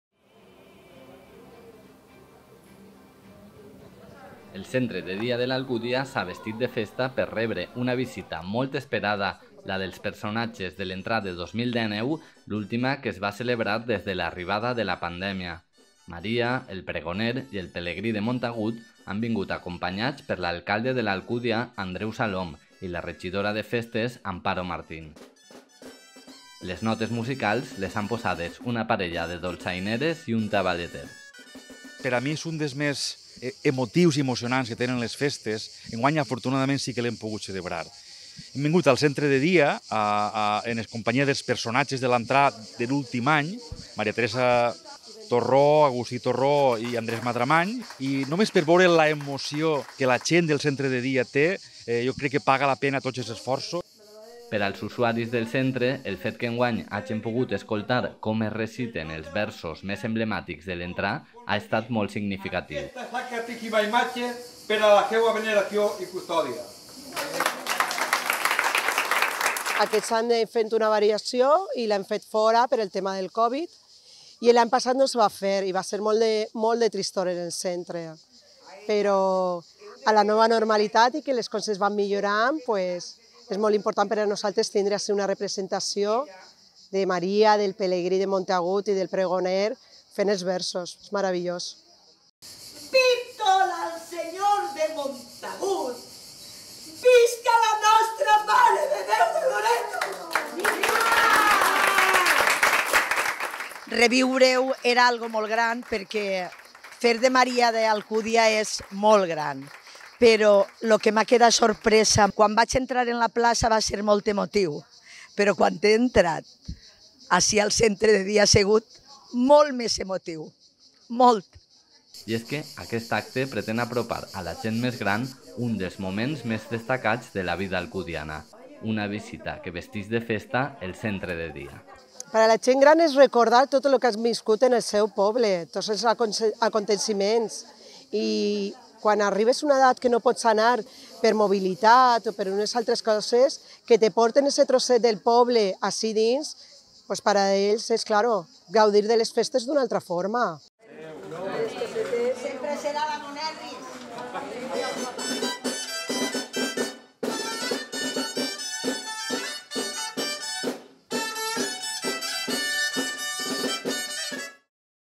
Visita dels personatges de l'Entrà al Centre de Dia
El passat dilluns 6 de setembre, l'alcalde, Andreu Salom i la regidora de festes, Amparo Martín, van visitar el Centre de Dia acompanyats de Tabal i dolçaina i dels personatges de l'Entrà 2019. Van fer una visita als usuaris del Centre per recitar els versos de Maria (Ma Teresa Torró), Pregoner (Agustí Torró) i Pelegrí de Montagut (Andrés Madramany).